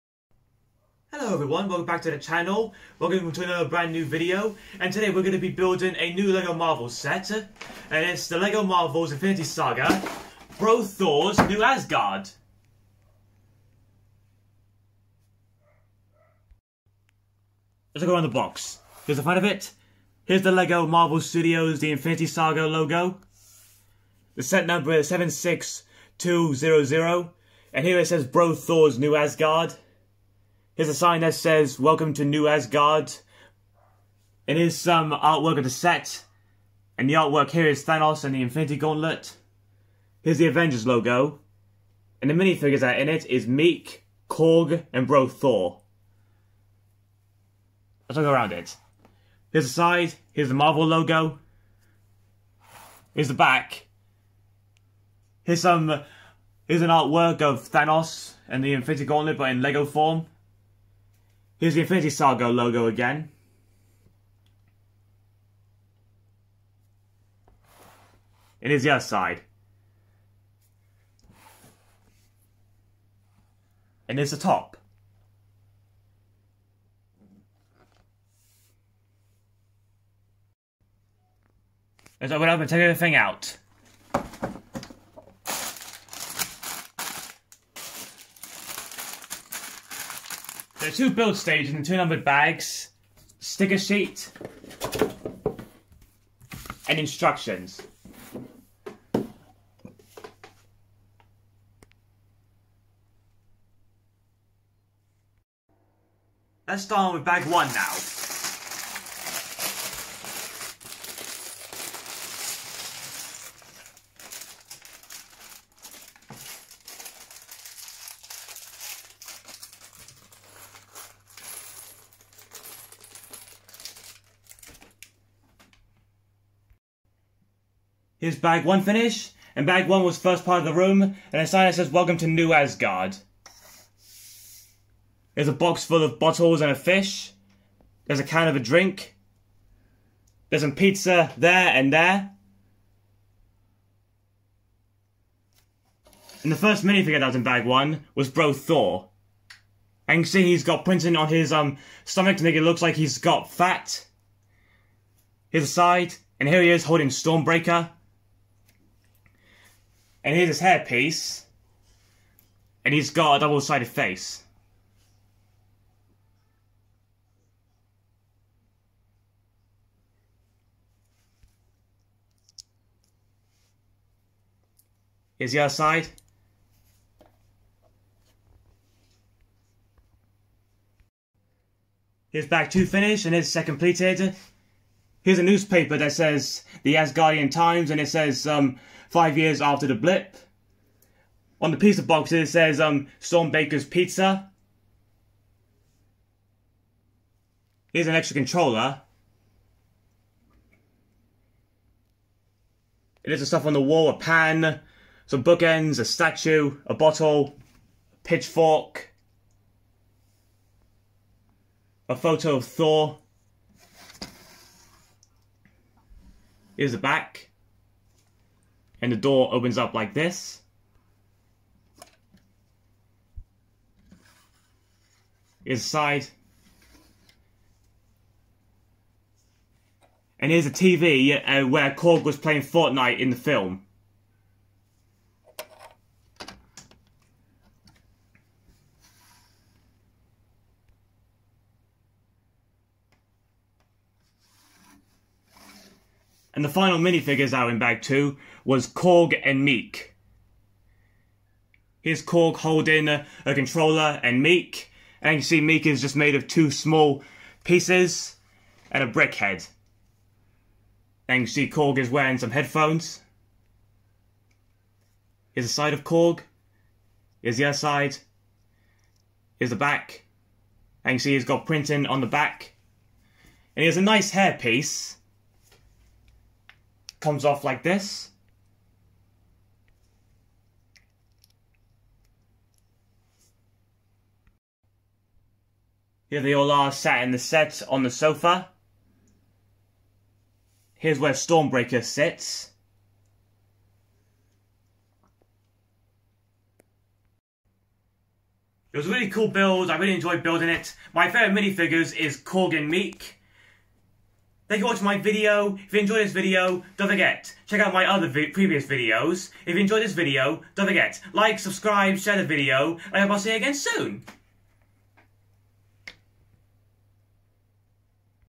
LEGO Marvel Bro Thor's New Asgard (76200)The Infinity Saga
Taking a look around the new Lego 76200 Marvel Studios The Infinity Saga set!!! Bro Thor's New Asgard set has some great details including Meik, Korg and Bro Thor minifigures

if you like this video or any of my others
Thank you for all those that have done so already